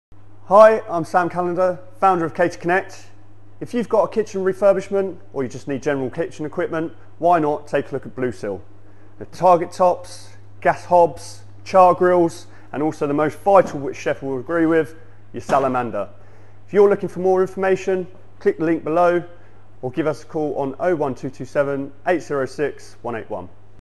Blue Seal Catering Equipment
Blue Seal offer a wide range of catering equipment within the hospitality market specifically aimed at the restaurant, hotels, education and health care sectors. This fantastic brand offers robust products at market leading prices, whilst also being visually attractive. If you are looking for a fryer, target top, cooktops, ovens and their best selling product the convection oven. Please get in touch today T: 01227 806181